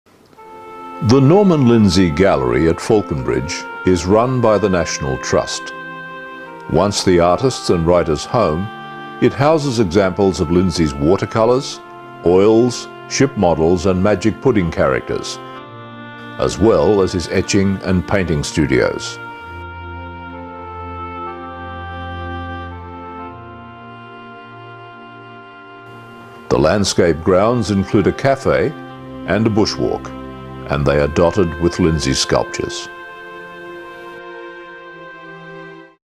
Norman Lindsay Gallery & Museum
Home of artist and writer Norman Lindsay until his death in 1969, this sandstone cottage and extensive grounds is now a gallery run by the National Trust displaying the artist`s watercolours, oils, etchings, ship models, and Magic Pudding characters.

Lindsay`s painting and etching studios are also open to view (regularly with a guided tour).

Landscaped grounds dotted with Lindsay's sculptures offer a delightful setting for picnics while Lindsay`s Café provides a unique dining experience in an idyllic bushland setting.